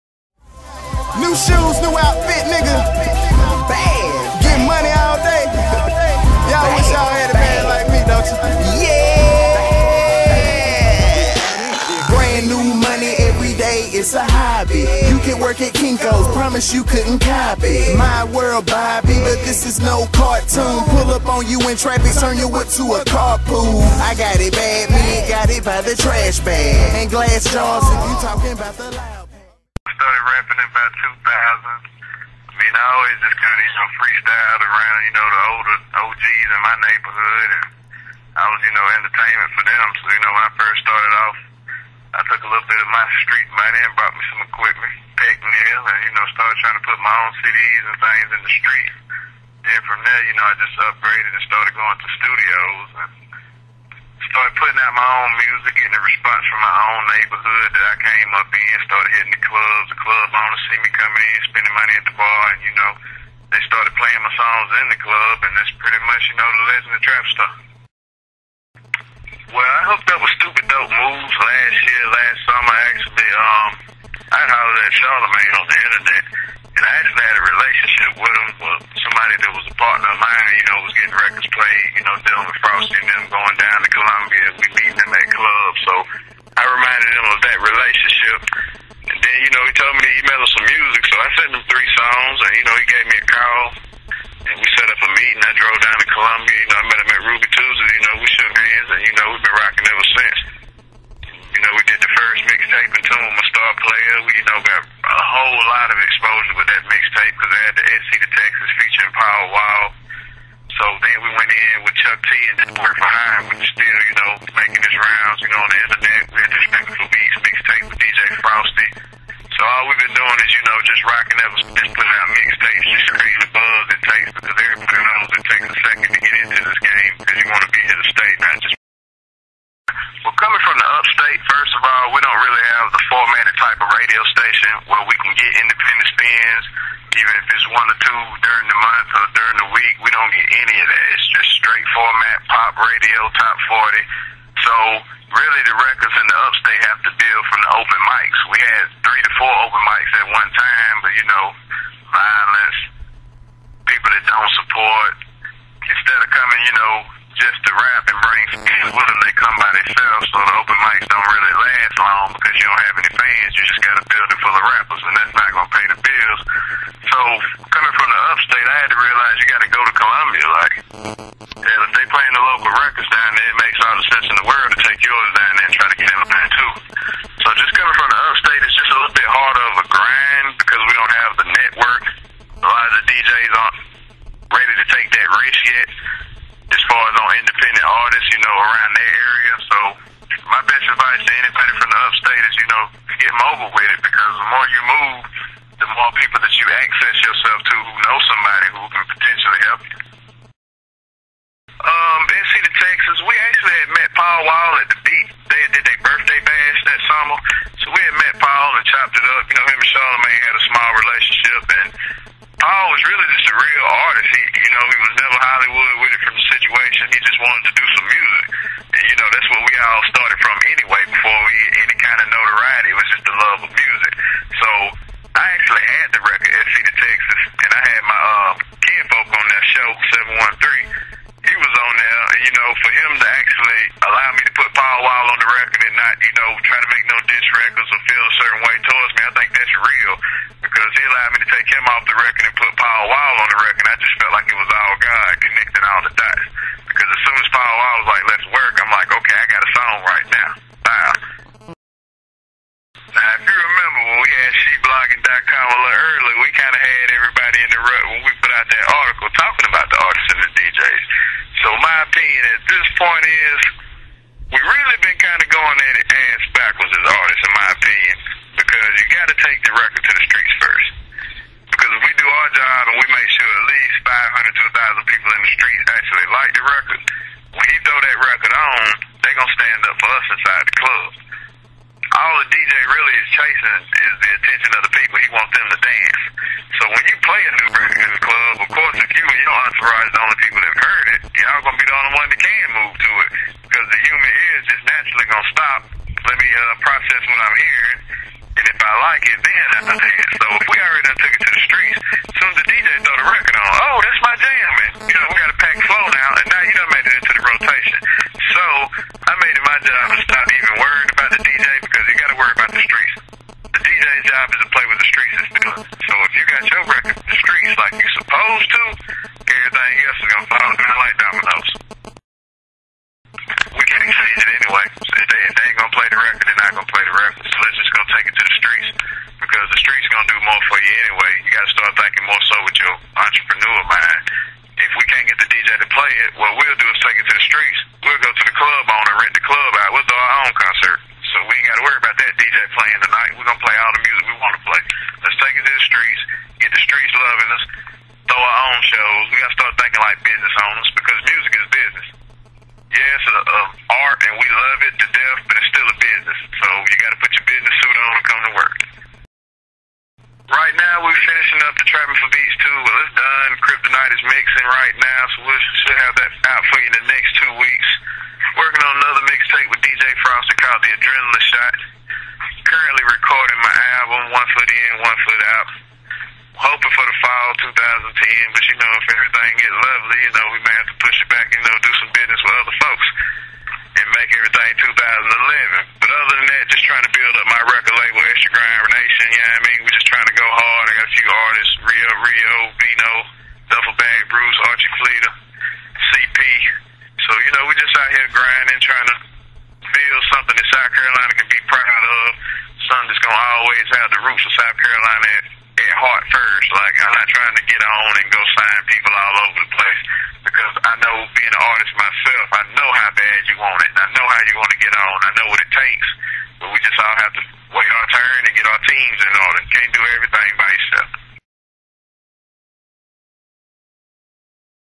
SheBloggin.com Interviews TrapStar
In part 5 of The Arrival interview series, I talked to SC star Trapstar about his music, linking with SDM, and the local scene.